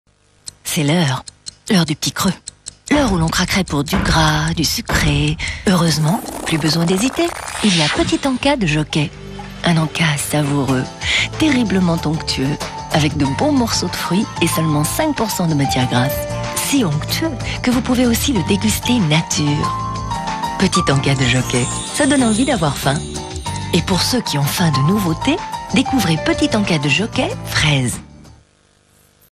Publicité 2001 Petit Encas De Jockey Danone (C'est l'heure du petit creux)
Publicité de 2001 pour le yaourt Petit Encas de Jockey de Danone.

Bon Visionnage.

Merci.